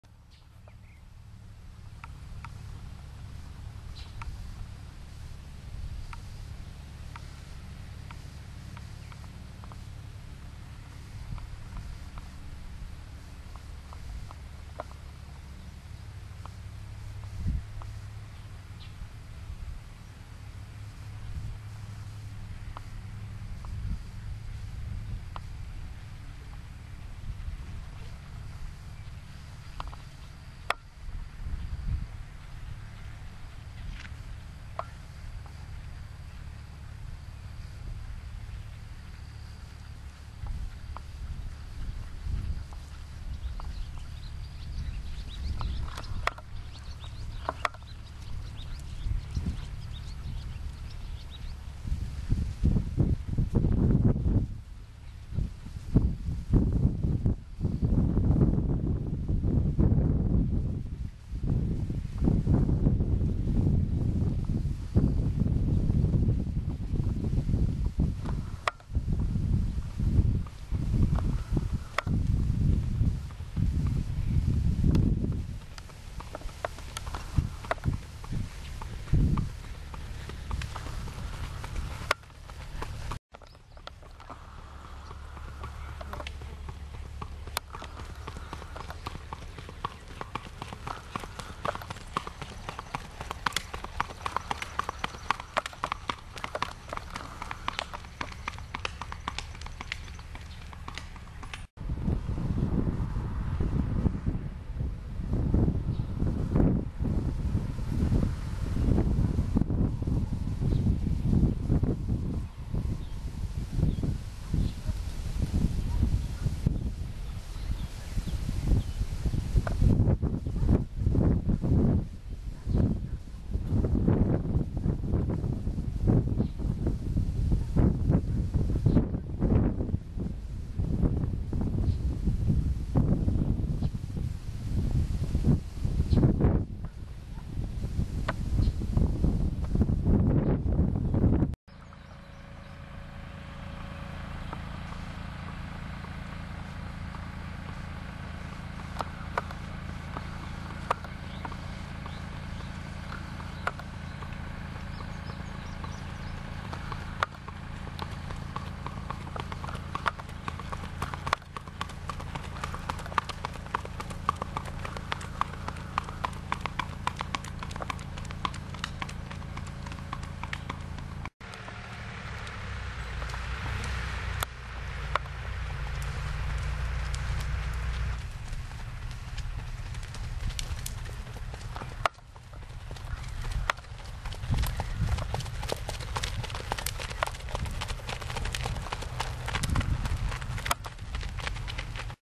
Bella- black Racking Horse mare
3 yr. old, 15h, silky smooth gait.